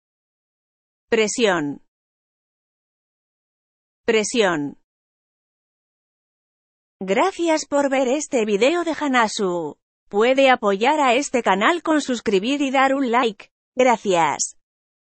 Como pronunciar 'Presión' en español
How to pronounce 'Presión' in Spanish.
Comment prononcer 'Presión' en espagnol.
Wie man 'Presión' auf Spanisch ausspricht.
「Presión」のスペイン語の発音の仕方